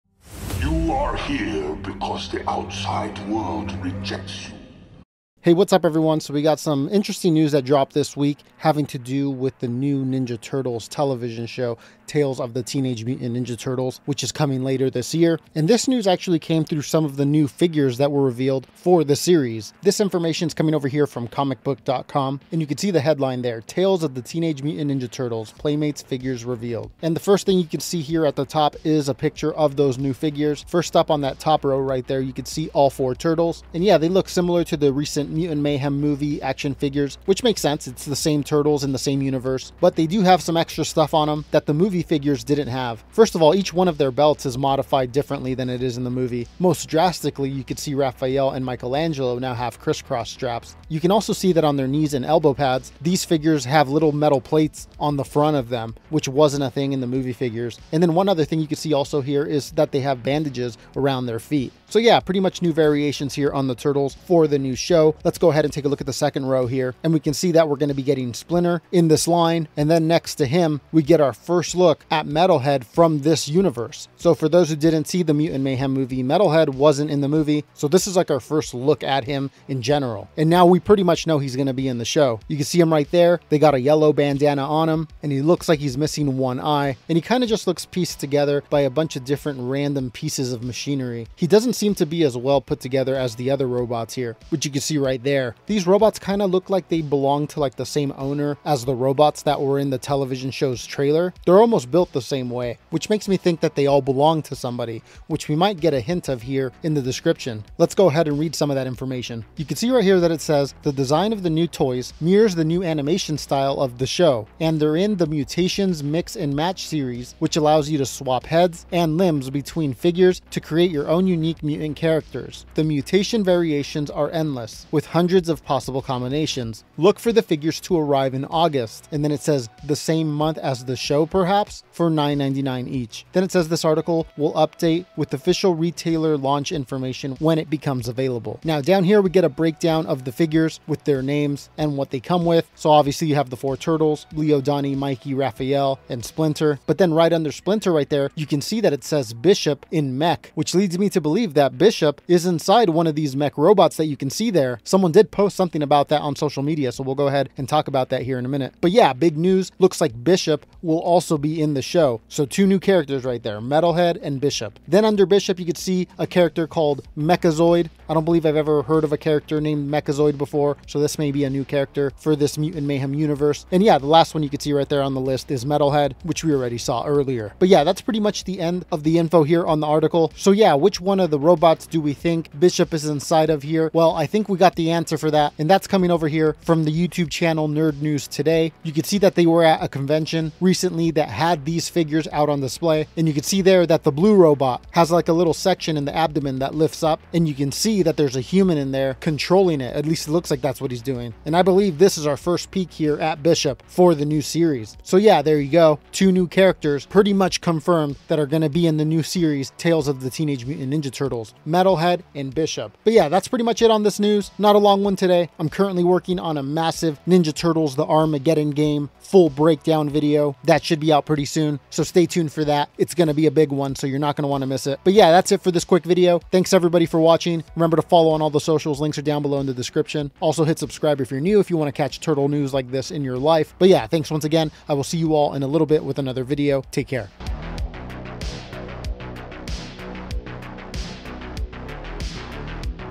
NEW Characters REVEALED For TMNT Show 2024?!
In this video, we take a look at some new figures for the upcoming series Tales of The Teenage Mutant Ninja Turtles. The figures reveal some new characters that will appear in the new TMNT show.